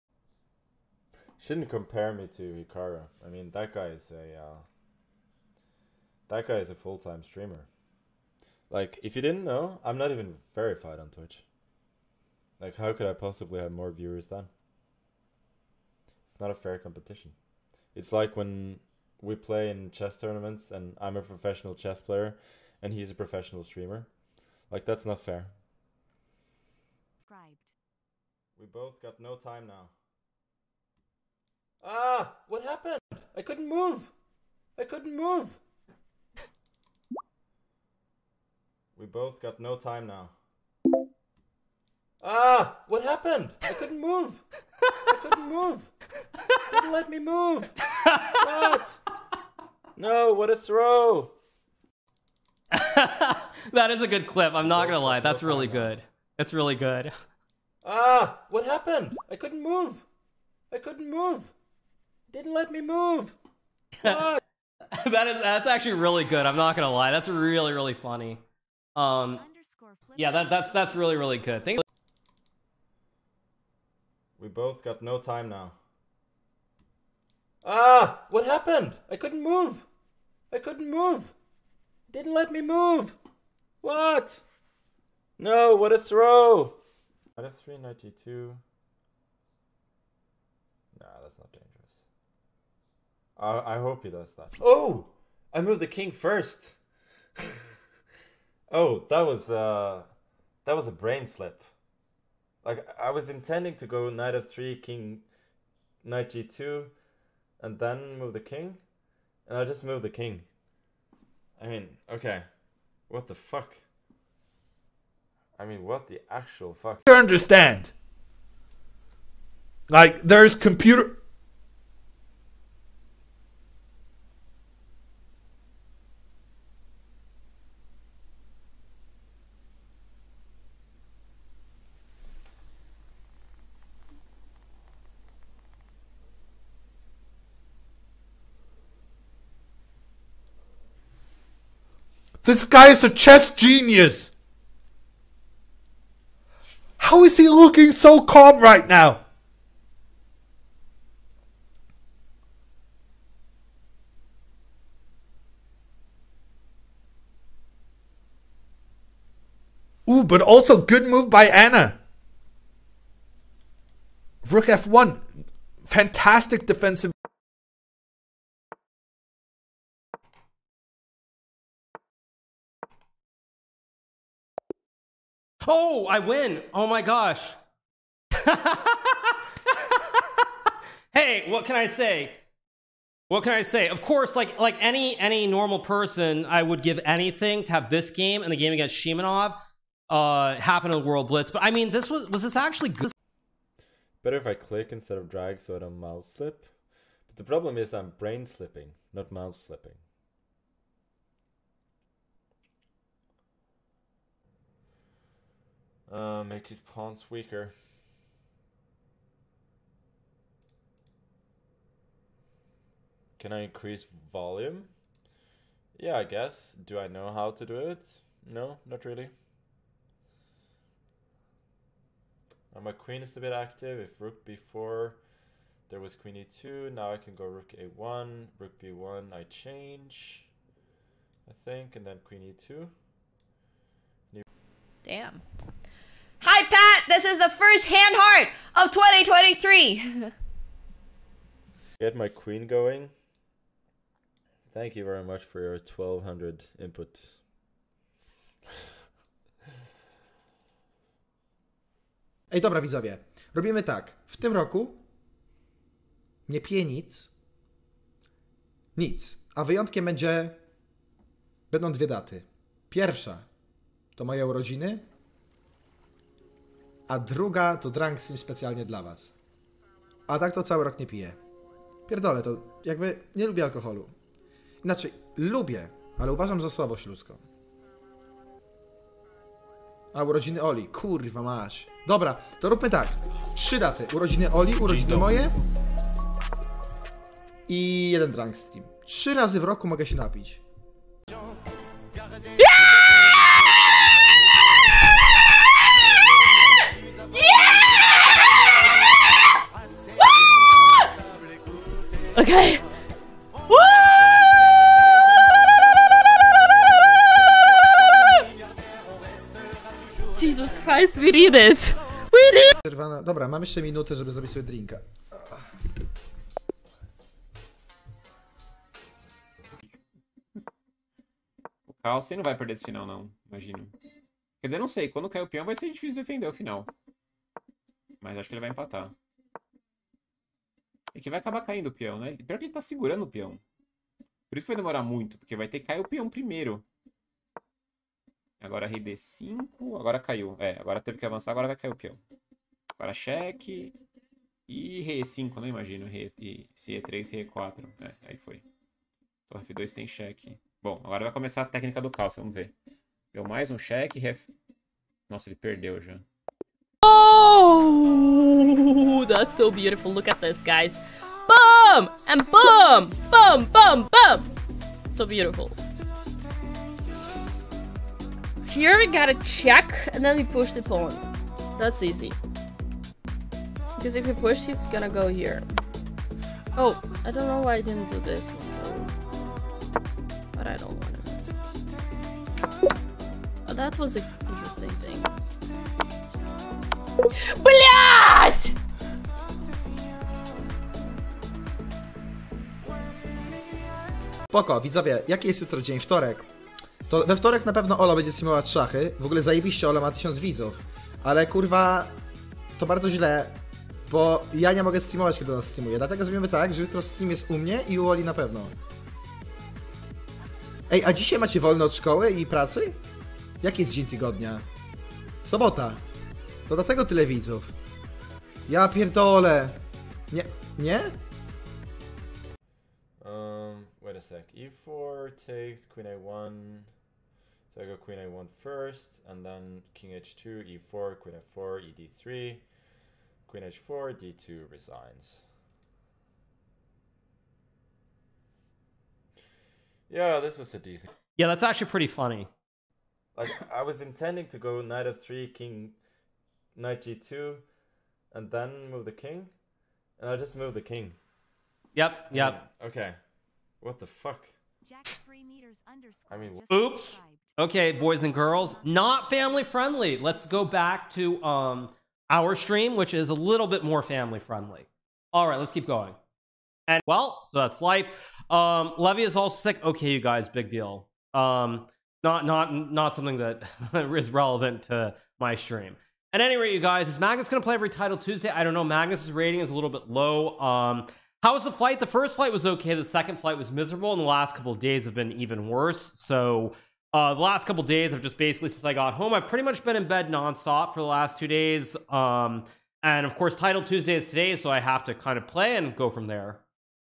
Don't compare Hikaru and Magnus (maskenissen) | Chess Highlights
Timestamps & vods:
[00:00] #1 maskenissen: Don't compare Hikaru and Magnus

[00:27] #2 GMHikaru: Hikaru reaction

[01:11] #3 maskenissen: STUCK IN MUD

[01:27] #4 maskenissen: Mouseslip

[01:56] #5 AnnaCramling: HAMMER'S REACTION TO QE2!!

[02:46] #6 GMHikaru: Hikaru vs Carlsen endgame

[03:12] #7 maskenissen: He is just like me fr

[03:24] #8 maskenissen: FORMER DRUNKSTREAMER IS STILL REELING FROM NEW YEARS

[03:54] #9 akaNemsko: Excited for 2023!!

[04:01] #10 maskenissen: Thank you for your 1200 input!

[04:12] #11 xntentacion: STOPALKOHOLOWI 0/3

[04:51] #12 TheBelenkaya: Pure happiness

[05:12] #13 xntentacion: 10/10 timing

[05:19] #14 GMKrikor: Carlsen não vai perder o final

[05:56] #15 TheBelenkaya: Ups and Downs of Puzzle Rush

[06:34] #16 xntentacion: dedukcja kreta

[07:02] #17 maskenissen: Magnus Calls a Short Resigning Line

[07:20] #18 GMHikaru: 6/7 Magnus vs Hikaru in Titled Tuesday? | !newvid !join !kittens

[07:46] #19 GMHikaru: Titled Tuesdays in 2023!| !newvid !join